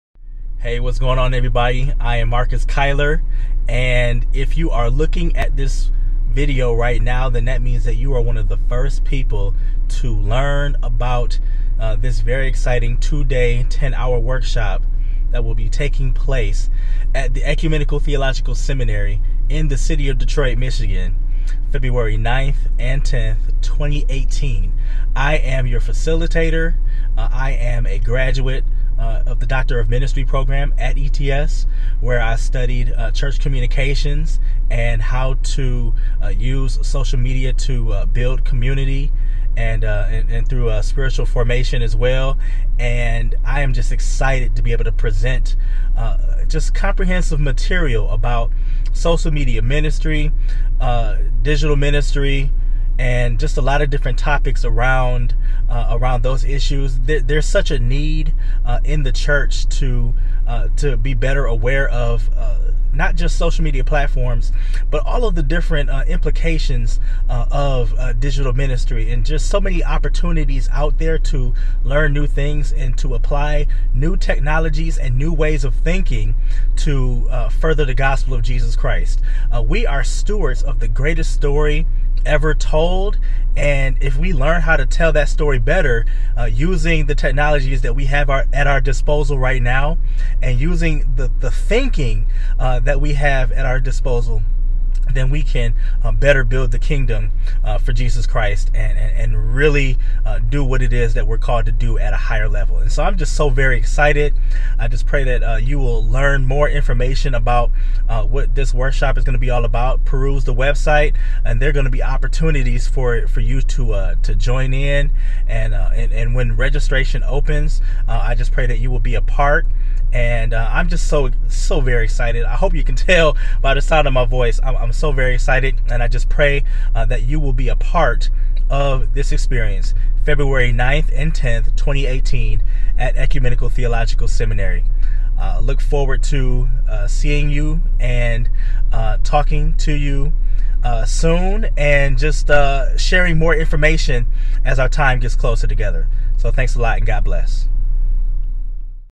“Can You Hear Me Now?” Workshop Promo #1
This is the first video announcing “Can You Hear Me Now?” the two-day social media ministry workshop to be held February 9-10, 2018 at the Ecumenical Theological Seminary in Detroit, MI. [recorded Thursday, November 9, 2017]